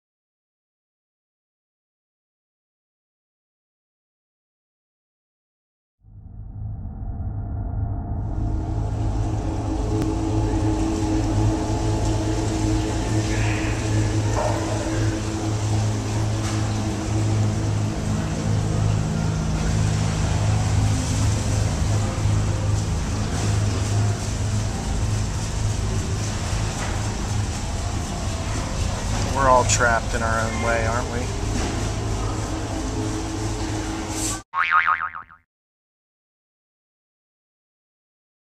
Abyssed Off
I was having a great day at the science center with Joseph, Nate & Sophie, but suddenly the flesh fell away and and that damn skeleton showed up. Again.